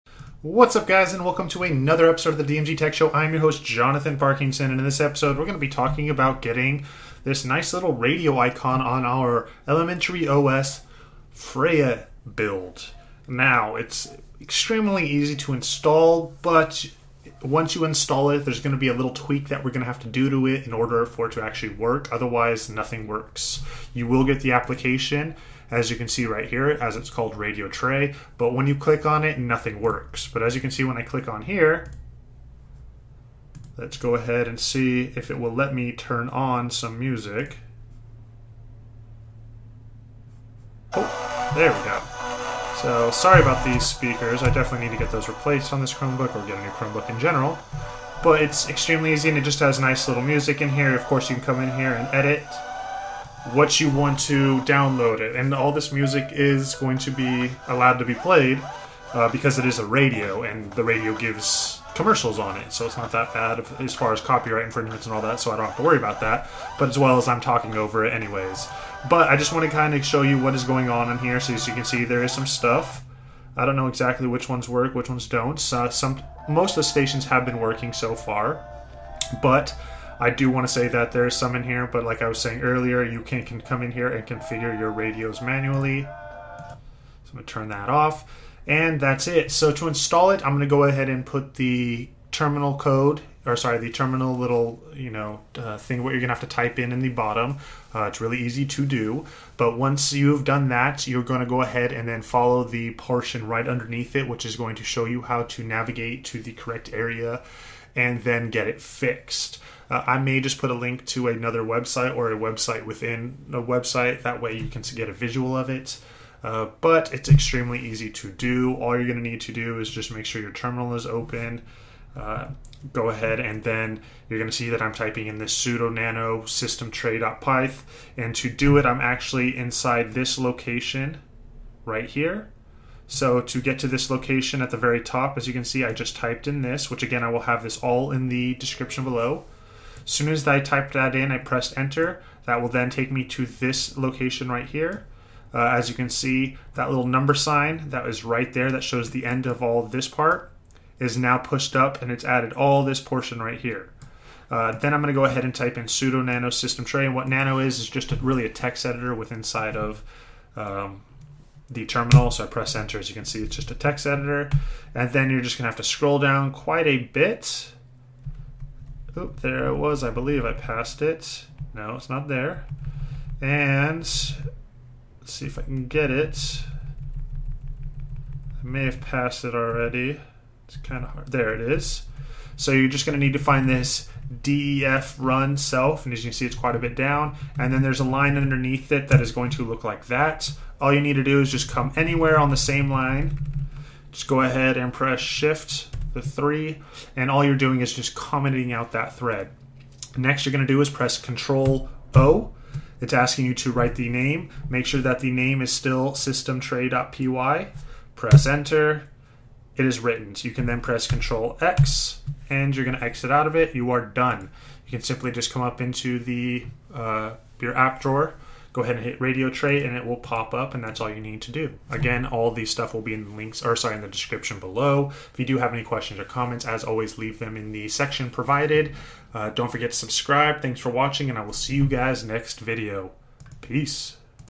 Elementary OS 101 Freya: Installing The Radio Tray -TechByDMG.com
The Radio Tray is a free application that sits beautifully in the Wing-panel of Elementary OS Freya. To Install it type in the following command in your terminal. Once you have done that you will need to follow the next steps to fix the issue with it not working right away.

To Install type this in the terminal:
sudo apt-get install radiotray

then stay in the terminal and type this:
cd /usr/lib/python2.7/dist-packages/radiotray

then:
sudo nano SysTray.py

and now navigate all the way down until you see this line: gtk.gdk.threads_init()

place a pound sign in front of it like I showed in the video so it looks like this:
#    gtk.gdk.threads_init()

press Ctrl O(not zero)
press enter
Press Ctrl X & open it from the apps drawer.

Visit Preferred Access Network for celebrity interviews, tech news and more.